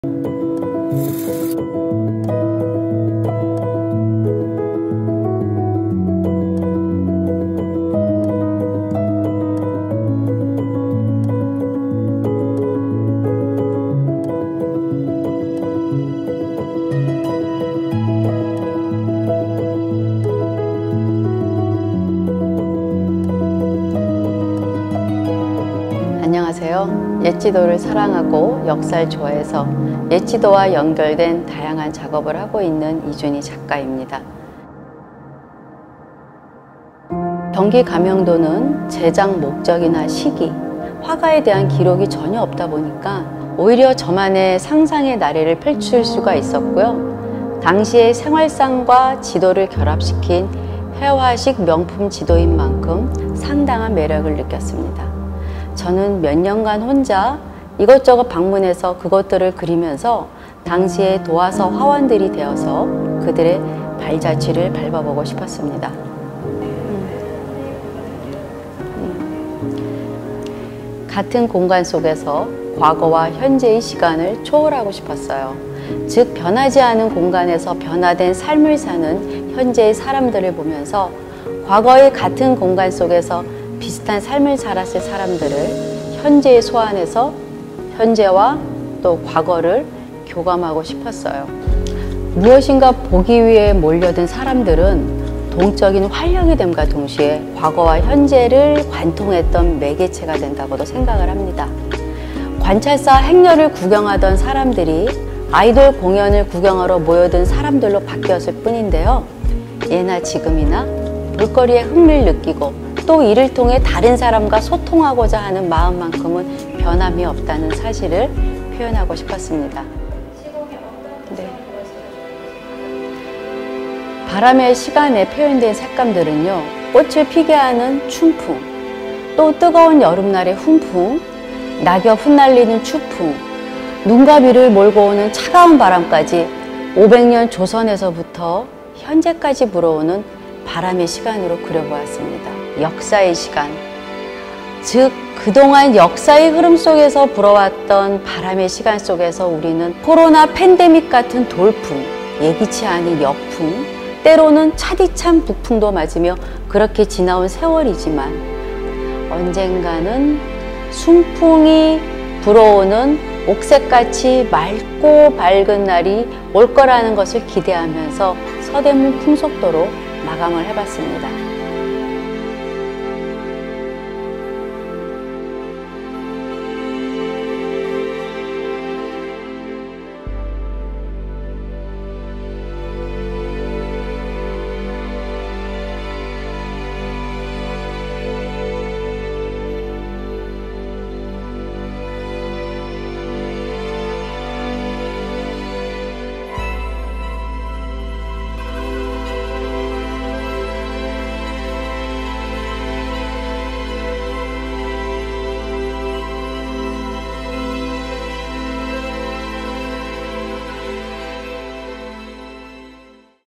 Artist interview with Lee Jun Hee 이준희 작가 인터뷰
바람의 시간 Time of the wind
이준희 15회 개인전 Lee Jun Hee 15th Solo Exhibition
다움 DaOoM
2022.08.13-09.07

Filmed by 153 Production